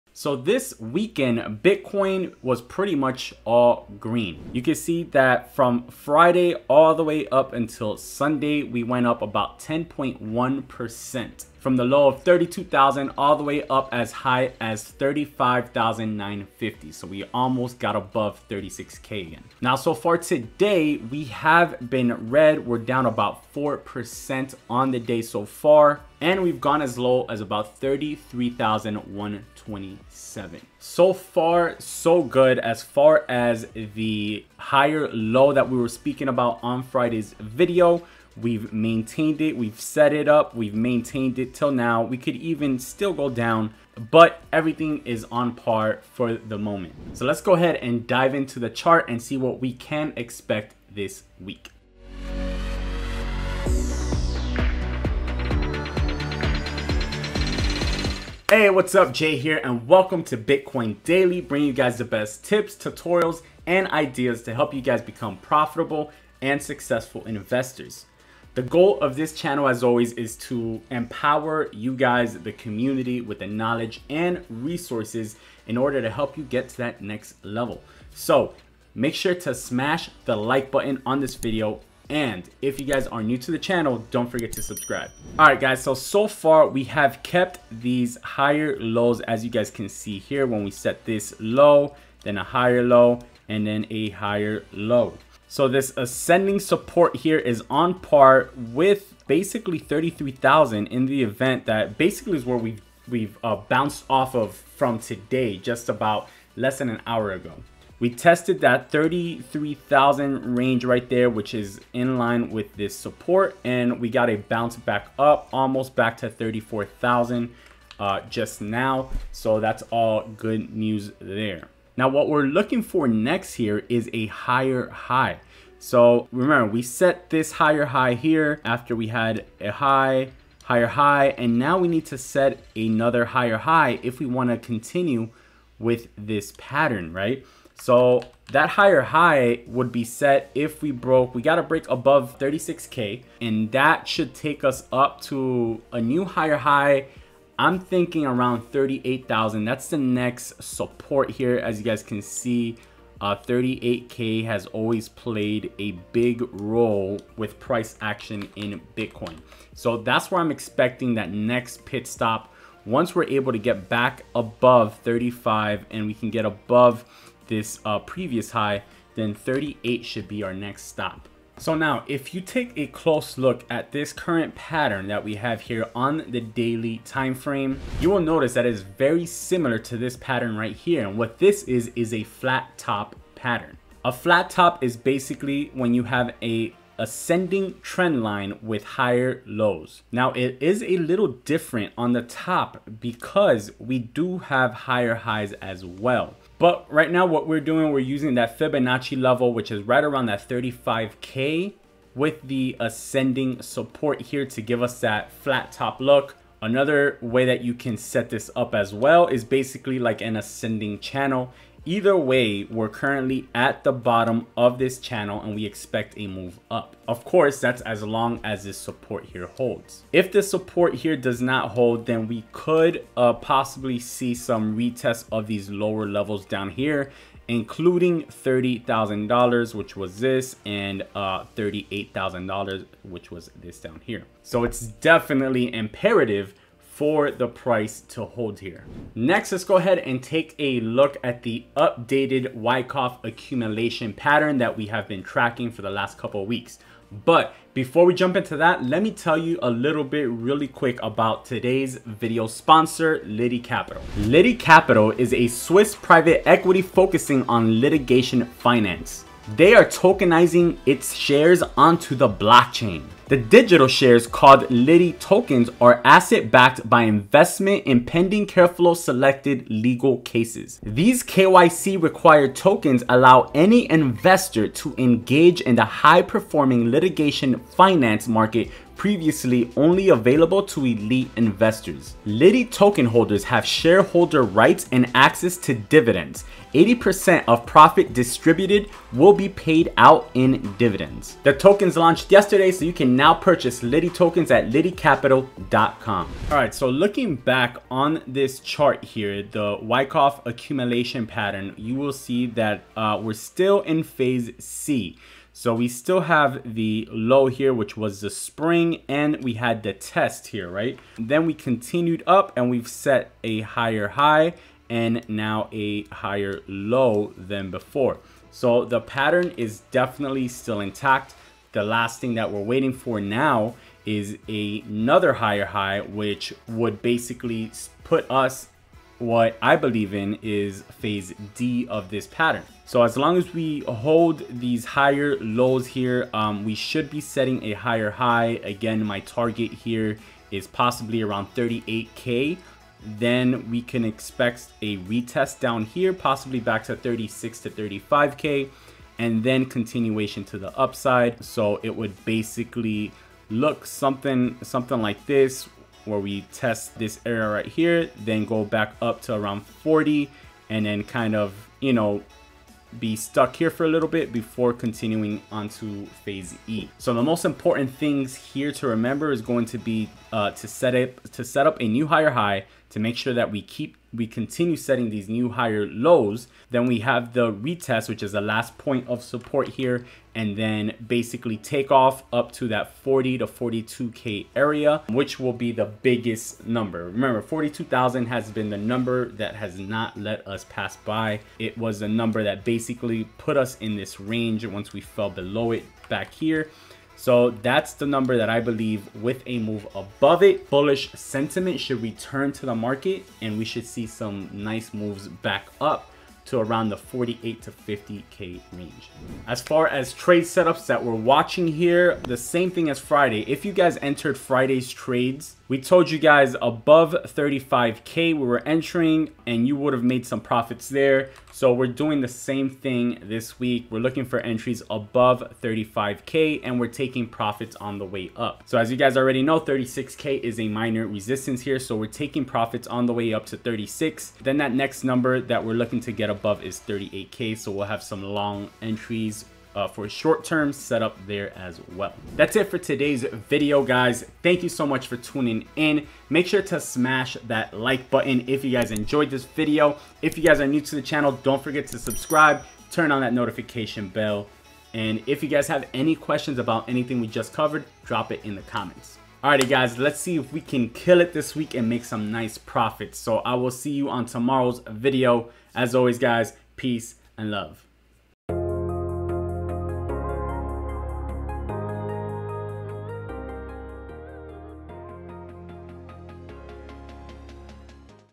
Bitcoin Price Analysis | Wyckoff Accumulation Schematic Update
In this video, we go over the weekend price movement and the outlook for the week!

⏰Time stamps⏰

Welcome: 00:00

Get paid 8.6% passively to HODL your Crypto on Blockfi! Use this link to get up to a $250 deposit bonus!